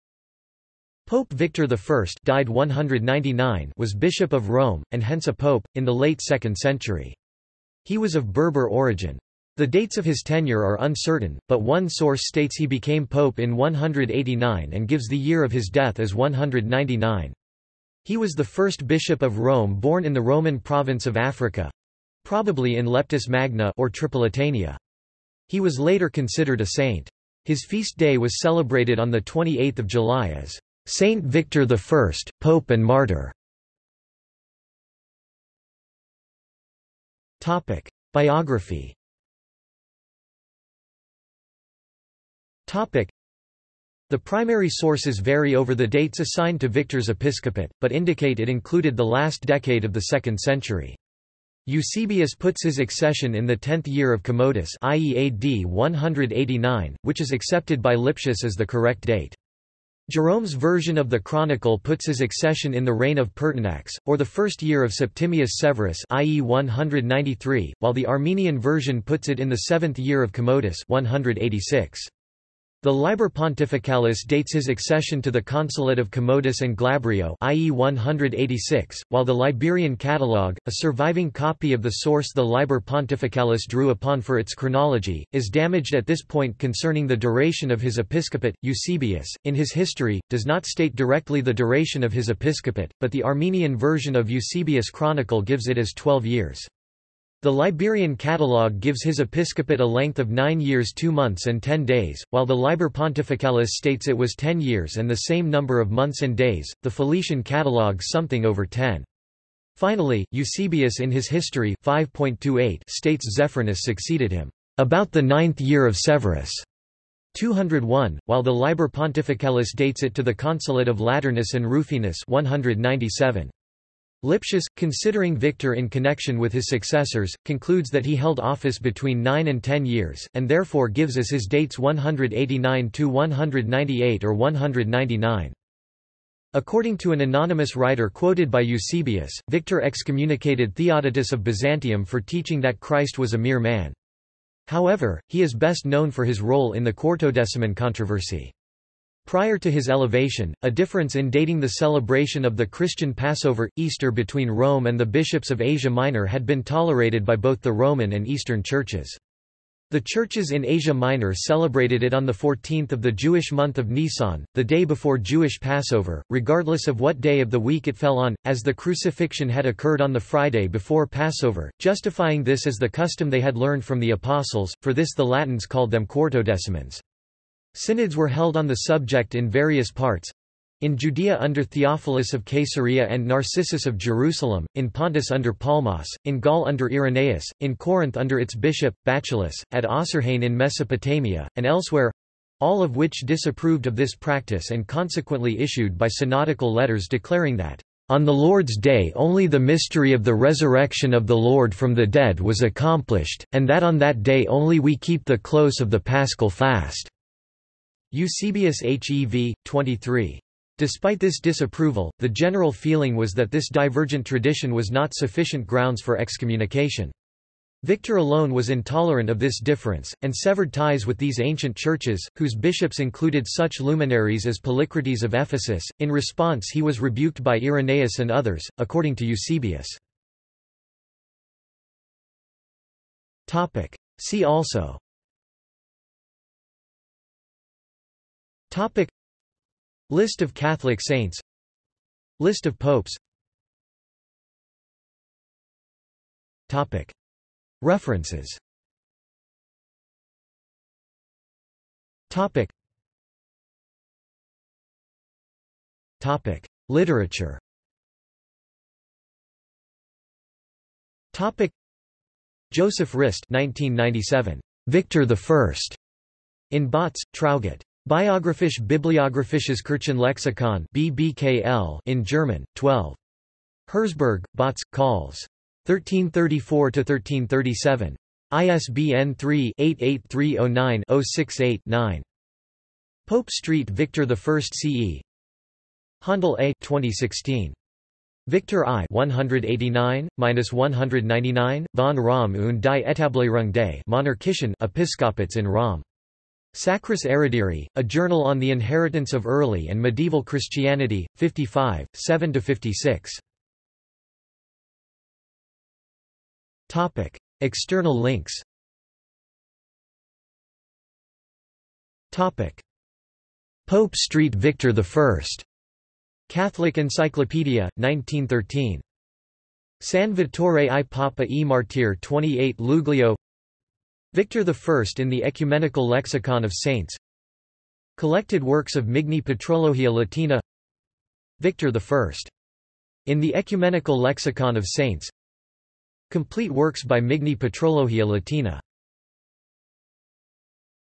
Pope Victor I | Wikipedia audio article
This is an audio version of the Wikipedia Article:
Pope Victor I

SUMMARY
Pope Victor I (died 199) was Bishop of Rome, and hence a pope, in the late second century. He was of Berber origin. The dates of his tenure are uncertain, but one source states he became pope in 189 and gives the year of his death as 199. He was the first bishop of Rome born in the Roman Province of Africa—probably in Leptis Magna (or Tripolitania). He was later considered a saint. His feast day was celebrated on 28 July as "St Victor I, Pope and Martyr".